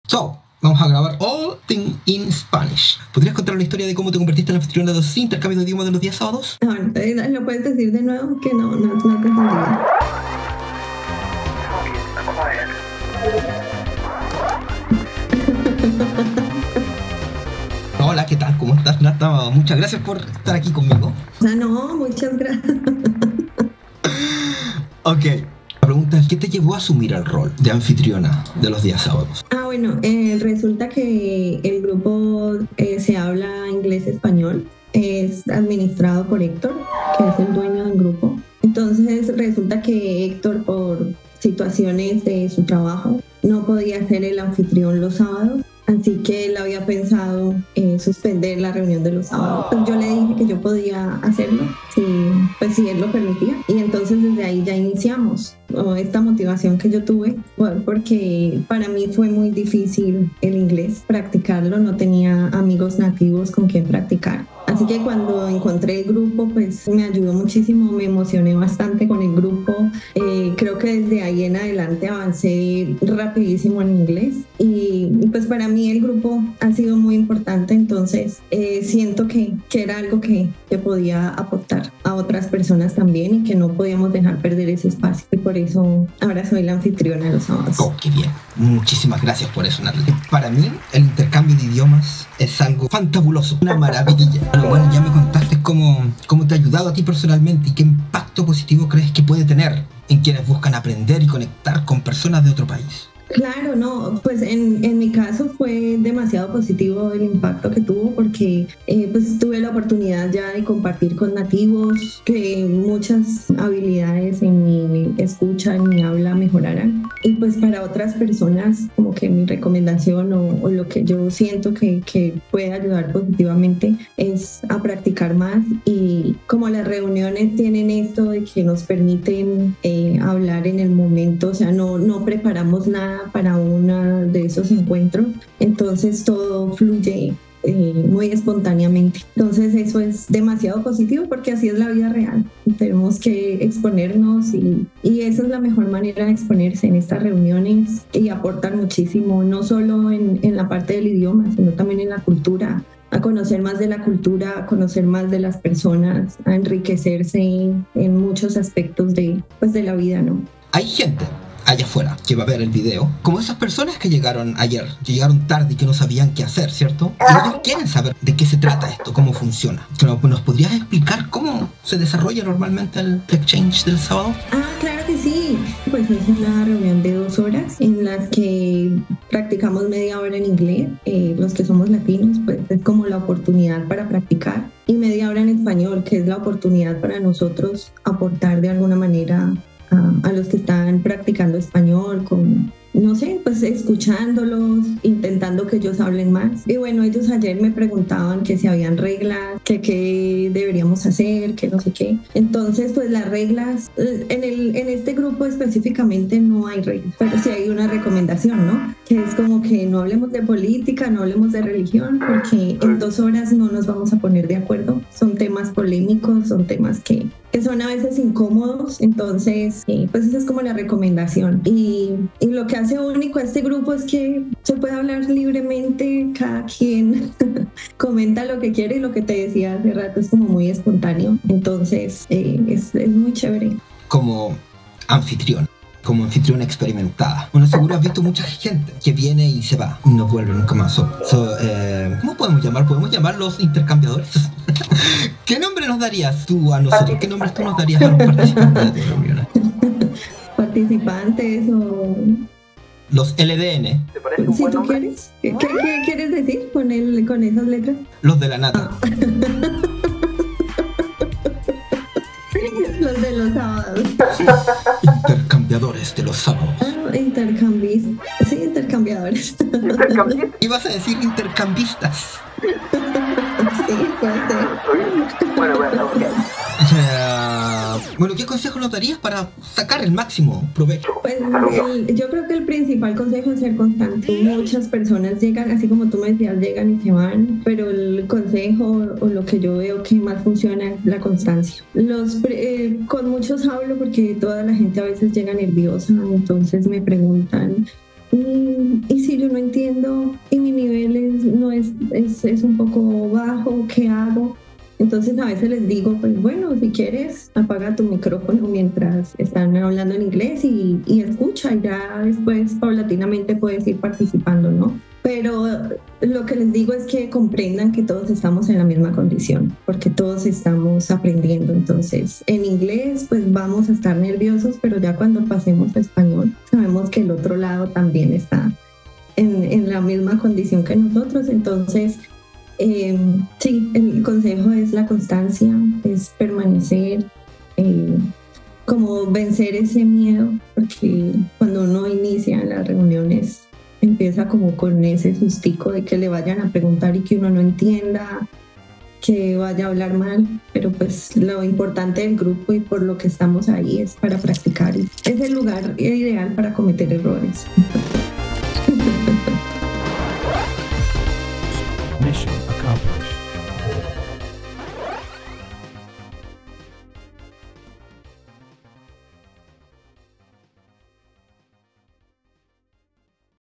🌟El heroico Secreto de Nathali para Dominar el Inglés! Lingoverse | The Game Changer
¡Amigas y amigos prepárense para sacar el máximo provecho de los intercambios de idioma de los sábados con las técnicas secretas que Nathali utilizó para dominar el inglés!
Sabías qué la valentía y pasión de Nathali salvó los intercambios de los sábados y aquí la tienes para que te lo cuente.
Entérate de las reglas, los trucos y su inspiradora historia para que tú también al igual que ella puedas dominar tu idioma meta.
Conectate con la asombrosa comunidad de "Se Habla" "Es el lugar perfecto para depurar tus errores" --Nathali 😎
¡Únete a Nathali todos los Sábados a las 1 pm, hora de Nueva York, y prepárate para un viaje lingüístico sin igual!🚀
El ciberespacio está lleno de recursos para practicar. En Lingoverse te traigo los que inspiran a alcanzar tus metas.